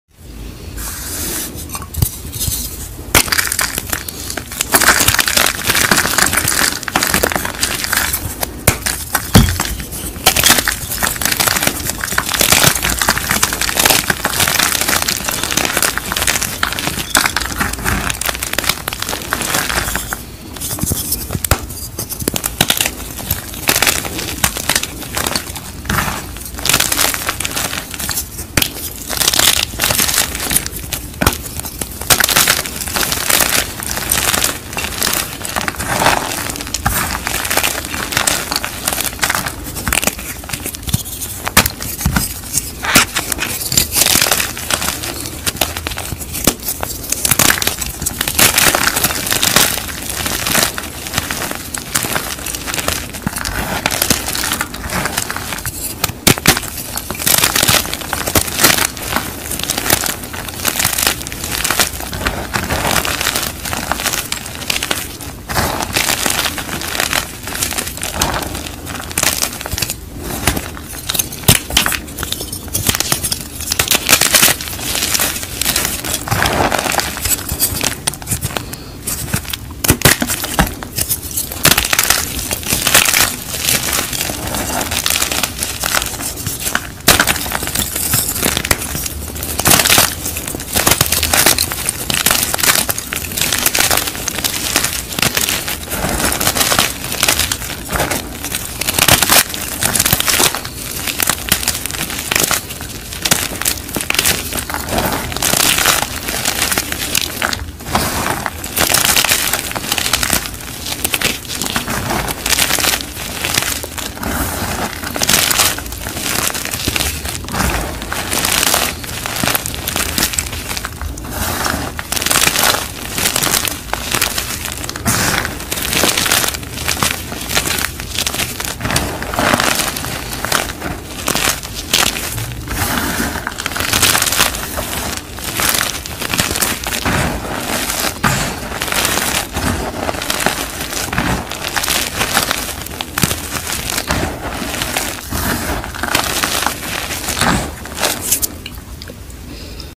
FreshBlockFriday❤️Super Crunchy Plain Jane crush/Grip & Gorilla grip/Repost/ASMR/Subscribe/
I repost this for freshblockfriday 😄
the first block is Gorilla grip and the other two are  Grip .. they are super duper Crunchy 😂 I hope u will enjoy this video 🤗

I'm not at home..so i don't have any prerecorded video because this is unplanned..😂
I will be back home maybe on Monday 👍

Hope everyone has a great weekend ❤️